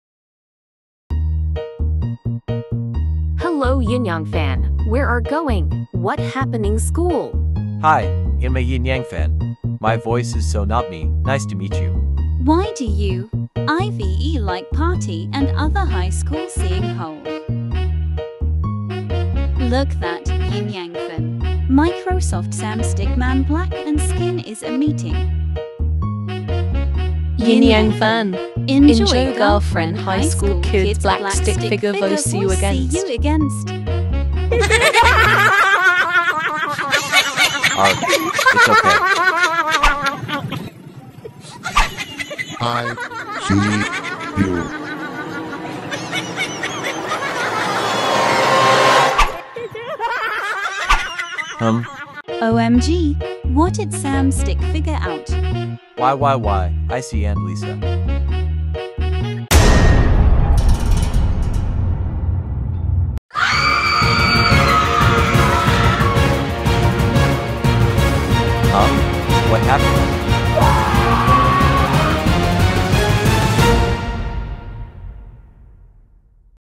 TANDACLB (2017): High school girl meets YinYangFan Dark Microsoft Sam just Stalker October 7th, 2023
Icy and Lisa meeting YinYangFan was old Microsoft Sam red Escaping YinYangFan.

Credit to @MrStickman3